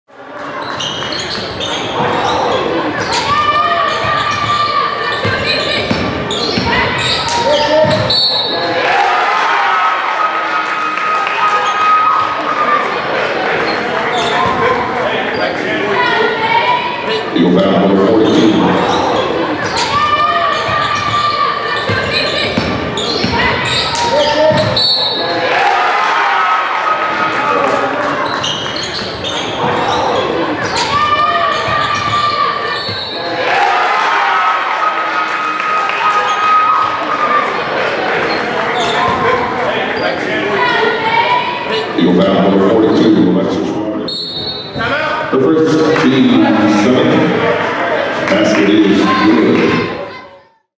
Jahnna Hajdukovich with the Trick Shot ~ AND1 ~ Alaska Basketball 2015
Jahnna Hajdukovich paced the Lathrop Malamutes to a lopsided win over the West Anchorage Eagles with a game high 14 points and earned Player of the Game honors in opening round play at the 2015 Dimond Lady Lynx Prep Shootout. Alaska high school basketball video highlights